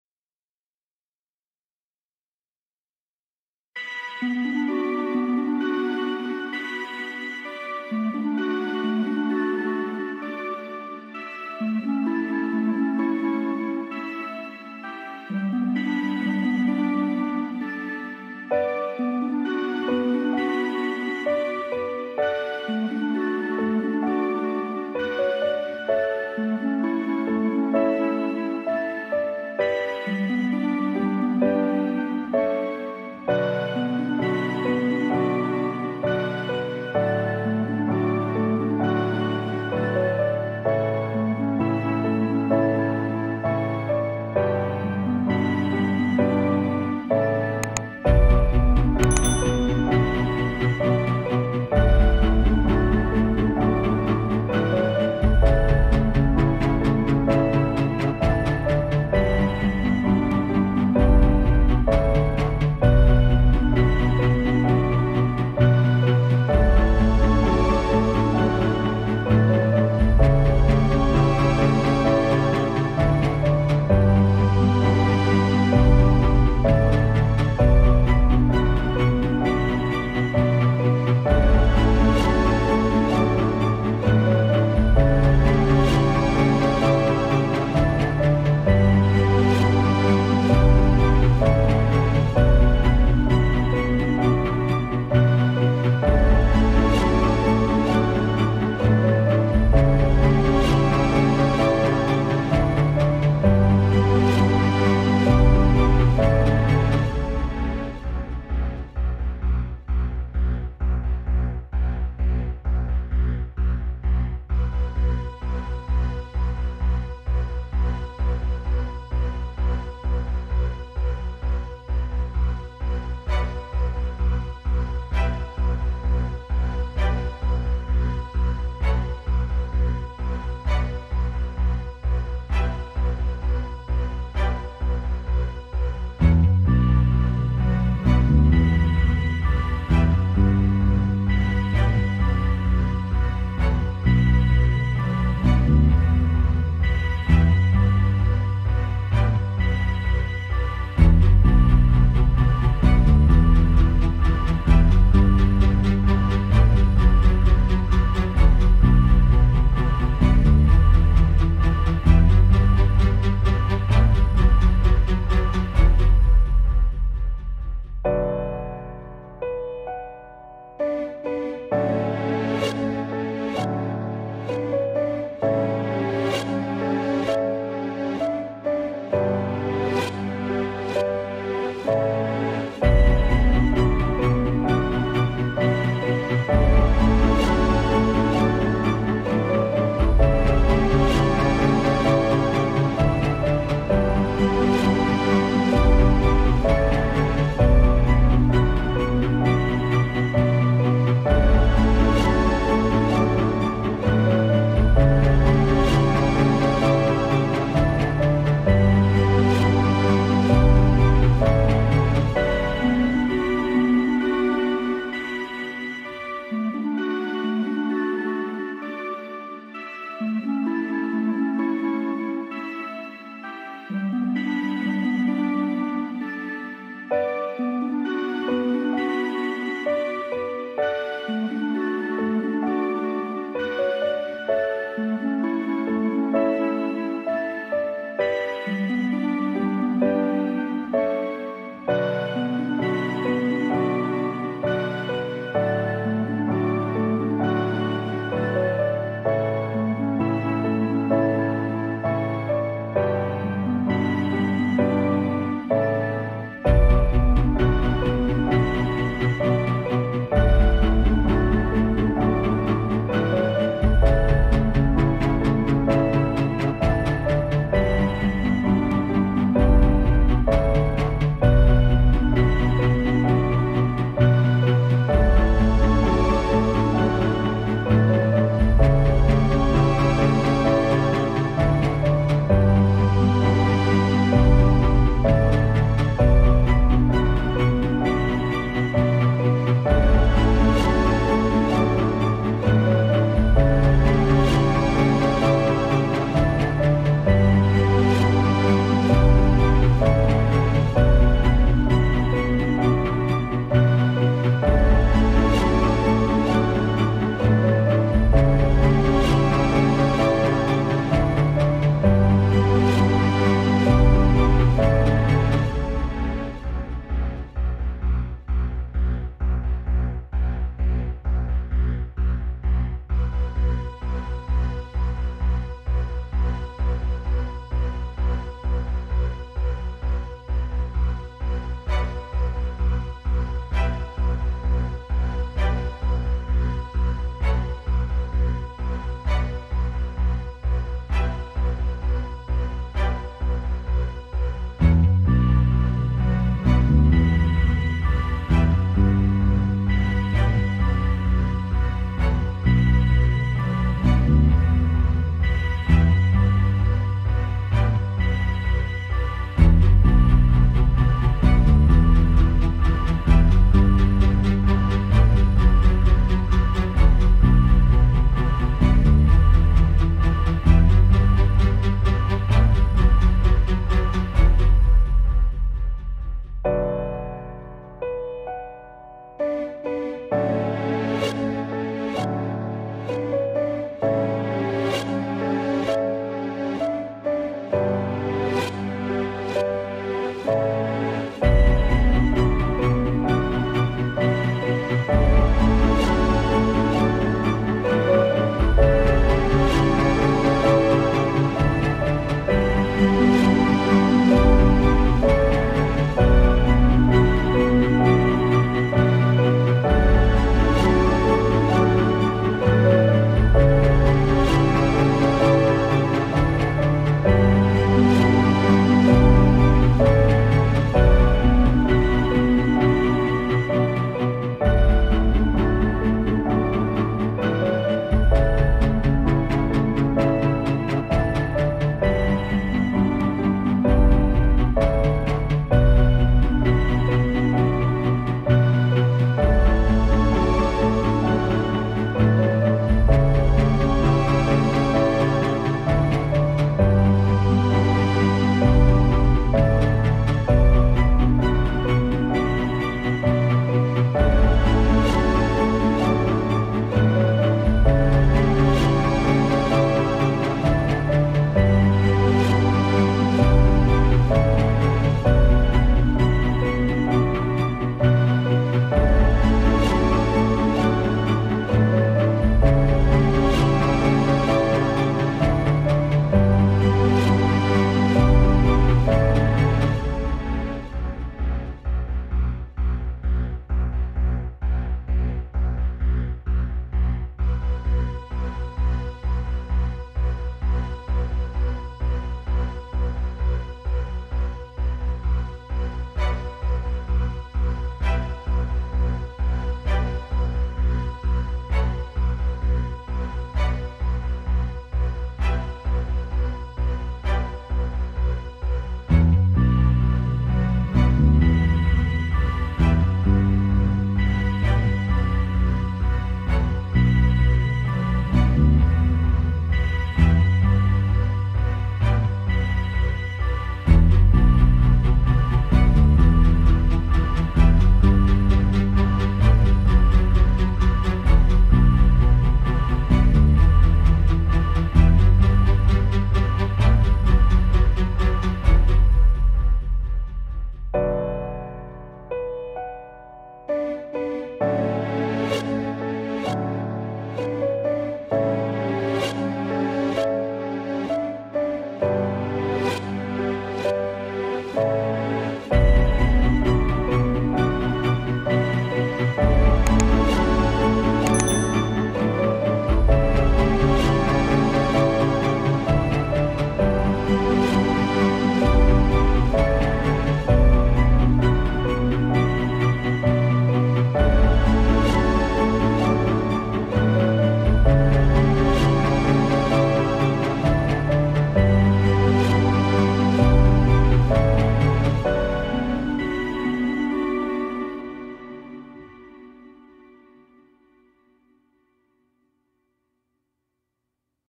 Ahsoka Tano Clone Trooper Helmet Lamp Hologram Plexiglass DIY Star Wars Clone Wars Acrylic
The design of the Ahsoka's clone trooper helmets from Clone Wars series is amazing, so I made a hologram type lamp from plexiglass.